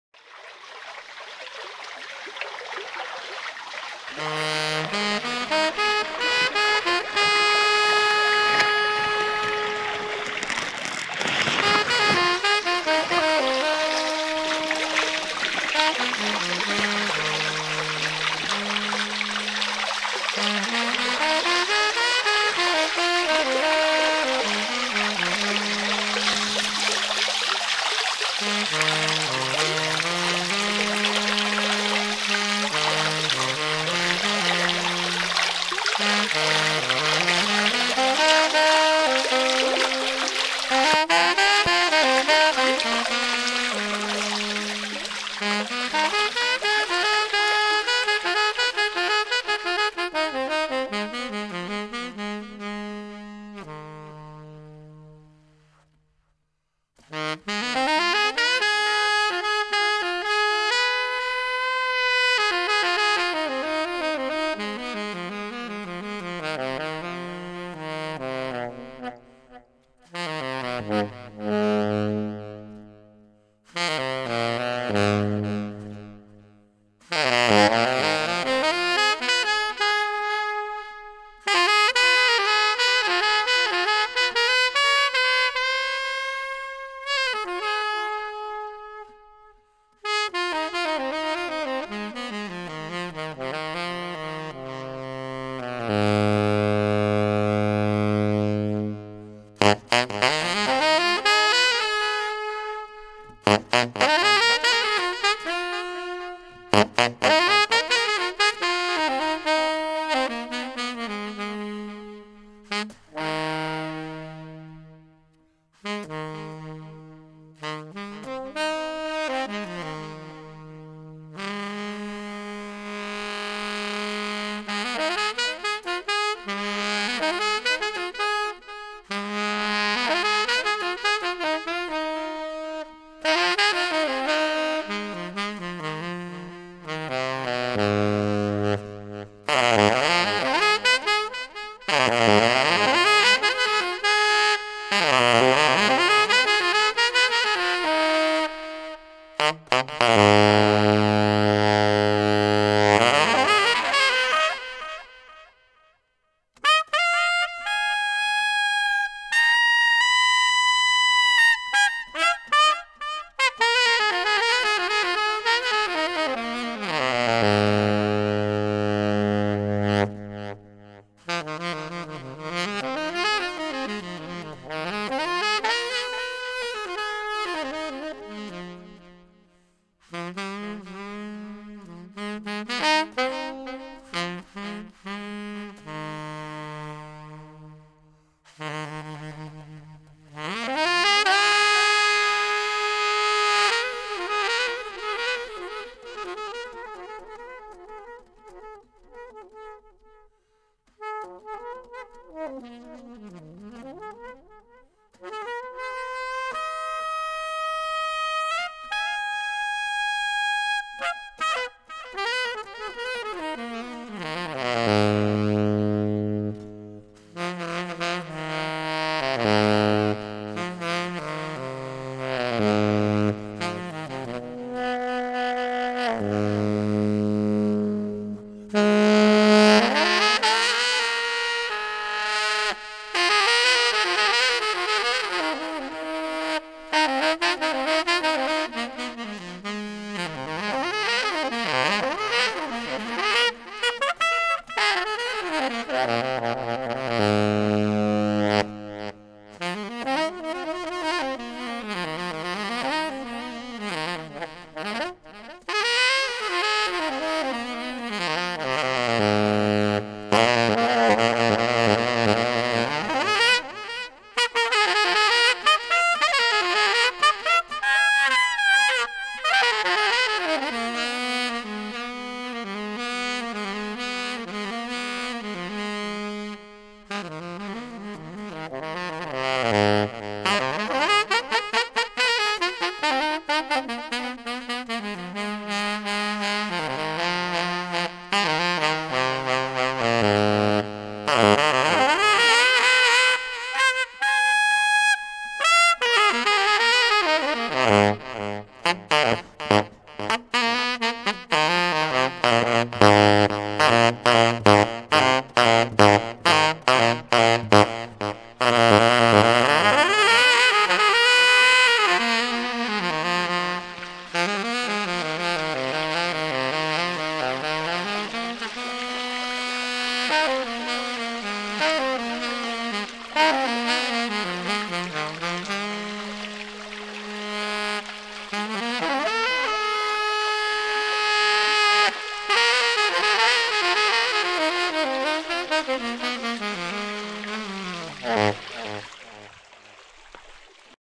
Spring Solo by Joe Cool Sax😎🎷
Here's a little backyard sax solo I thought would be fun and relaxing to listen to.